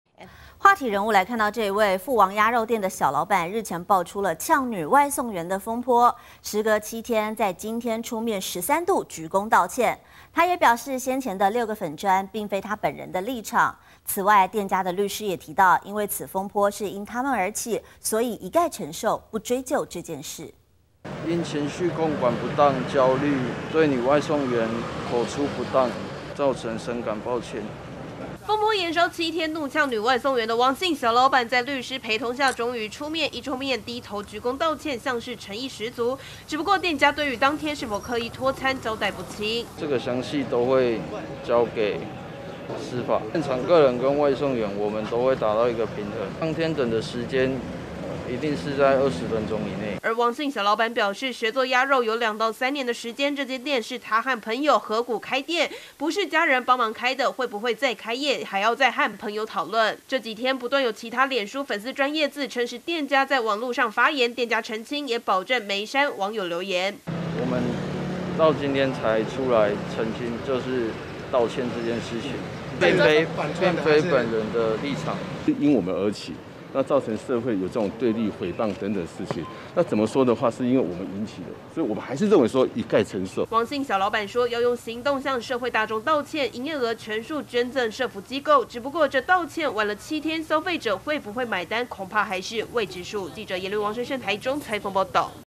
"粉專非本人立場" 鴨肉店小老闆急澄清
富王鴨肉店小老闆日前爆發出嗆女外送員風波，事隔7天後13度鞠躬出面道歉，他也表示，先前的6個粉專，並非他本人的立場。此外，店家律師也提到，因為此風波是因為他們而起，所以一蓋承受，不追究這件事。